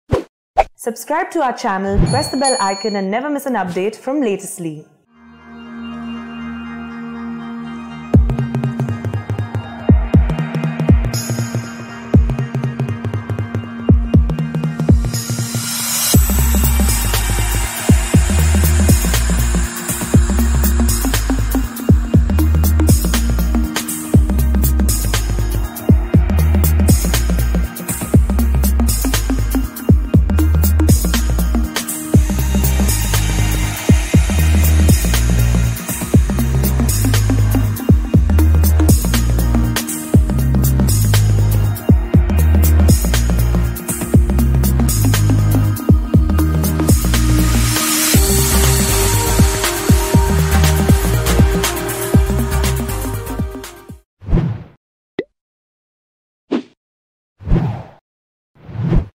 Good Friday 2022 Messages: Biblical Sayings, Images, Bible Verses & Quotes for Christian Holy Day
The Friday before Easter Day, on which Christians observe the commemoration of the Crucifixion of Jesus Christ, is marked as Good Friday. This year, the date of the Good Friday falls on the 15th of April. Members of many Christian denominations mark the day of grief with fasting and church services. To observe the day of Holy Friday, we have curated verses from Bible, hymns, quotes, sayings, thoughts and HD images that you send to your loved ones.